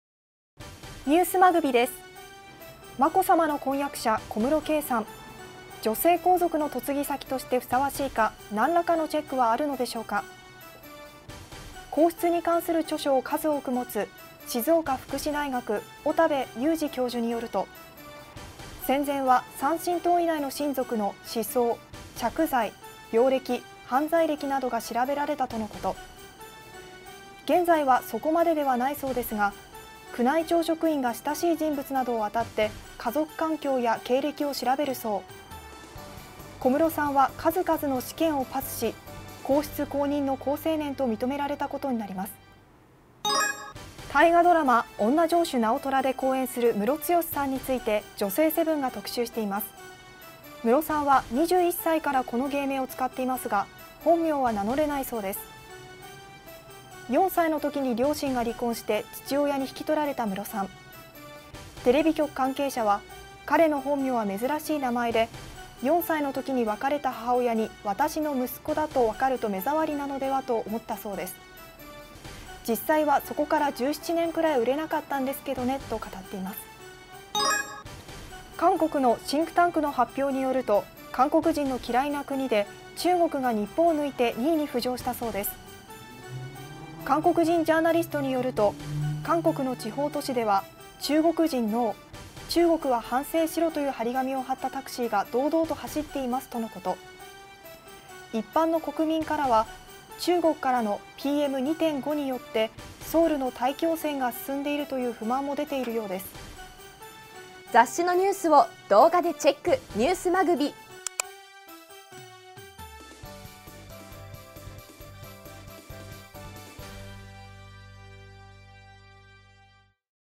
小室圭氏 数々の試験をパスし「皇室公認の好青年」の認定
◆『直虎』で好演のムロツヨシ「本名が明かせない」理由